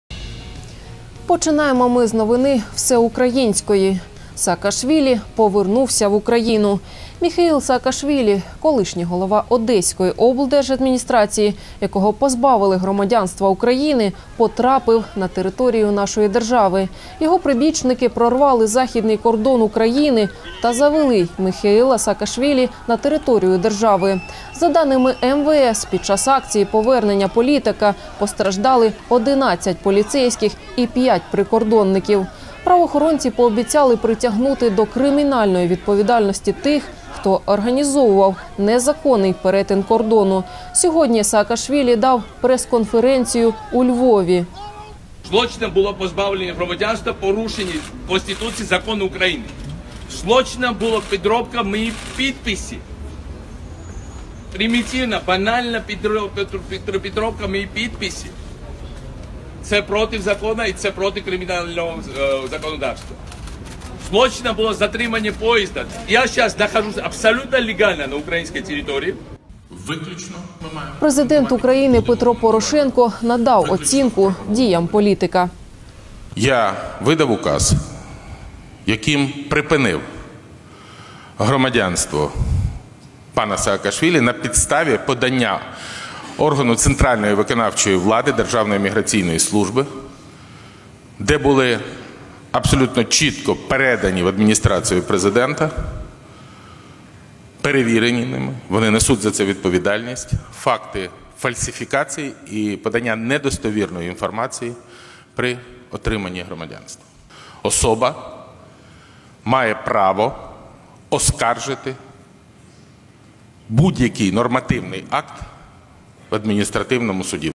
Резонансний перетин кордону Саакашвілі
Саакашвілі повернувся в Україну. Міхеіл Саакашвілі, колишній голова Одеської облдержадміністрації, якого позбавили громадянства України,  потрапив на територію нашої держави. Його прибічники прорвали Західний кордон України та завели Міхеіла Саакашвілі на територію держави. За даними МВС під час акції повернення політика постраждали 11 поліцейських і п’ять прикордонників. Правоохоронці пообіцяли притягнути до кримінальної відповідальності тих, хто організовував незаконний перетин кордону. Сьогодні Саакашвілі дав прес-конференцію у Львові.